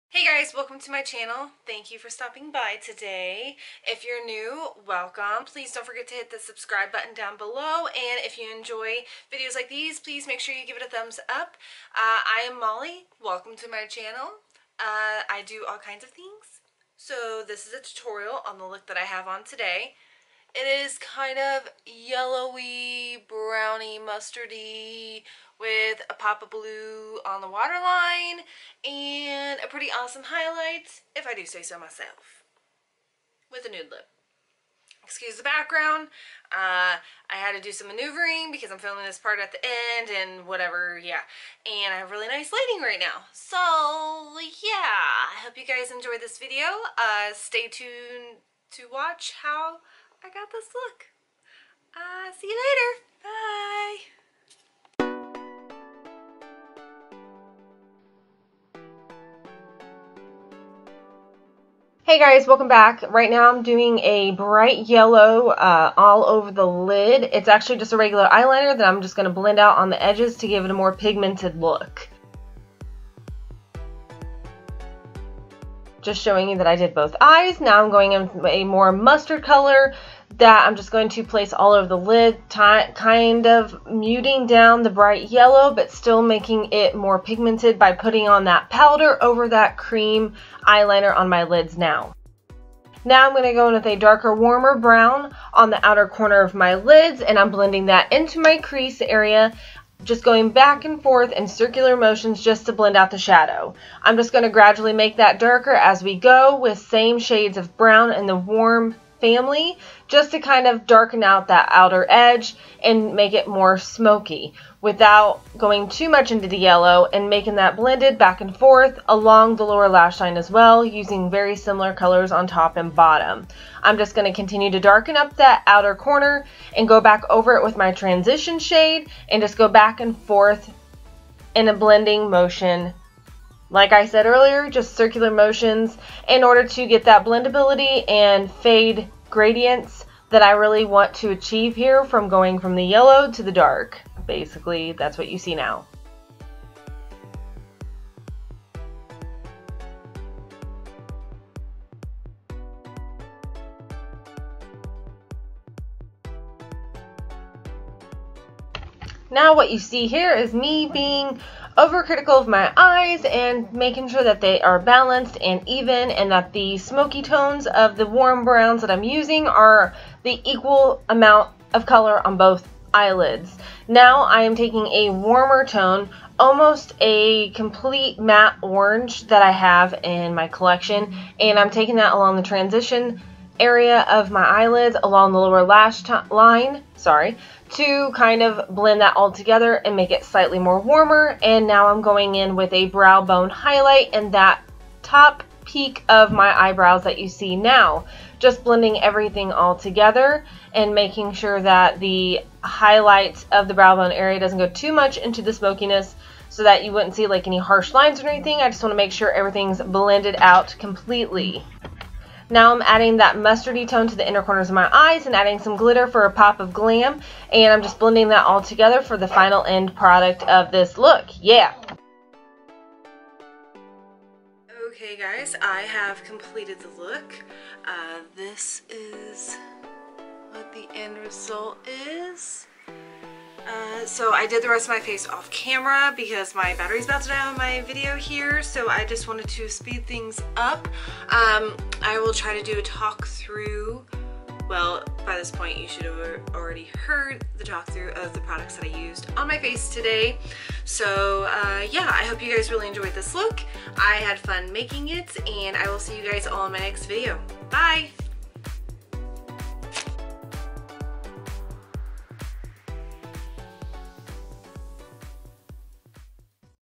Mustard Smokey Eye Tutorial - Miss Molly
Hey guys!!

Welcome back ! - This little video is all about the color yellow and not so yellow tones with some glitter thrown in ! - I really hope you all like the video and enjoy! -

I will see you all in my next video !

Make sure to like it and follow!!